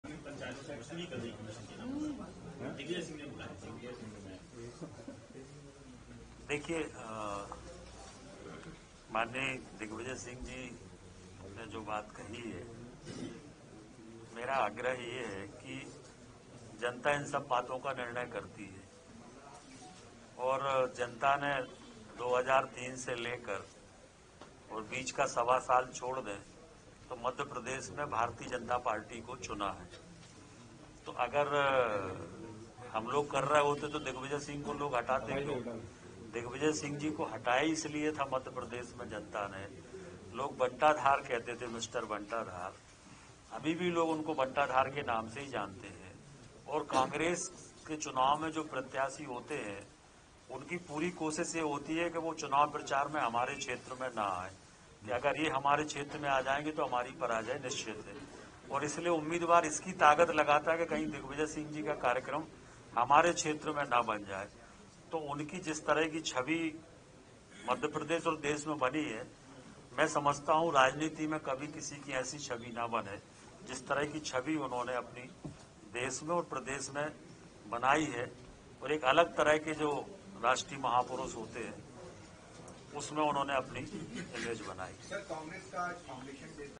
भोपाल.दिग्गी राजा के बयान पर क्या बोले मंत्री भूपेंद्र सिंह..
नगरीय विकास तथा आवास मंत्री श्री भूपेन्द्र सिंह ने पूर्व मुख्यमंत्री दिग्विजय सिंह के लिए कहा कि उन्हें 'मिस्टर बंटाधार' कहा जाता है..आज भी प्रदेश के लोग उन्हें यही कहकर बुलाते हैं..- चुनावों में कांग्रेस प्रत्याशी भी इस बात के लिए ही ताकत लगाते हैं कि कहीं उनके क्षेत्र में दिग्विजय सिंह का कार्यक्रम न बन जाए, वरना वह चुनाव हार जाएंगे..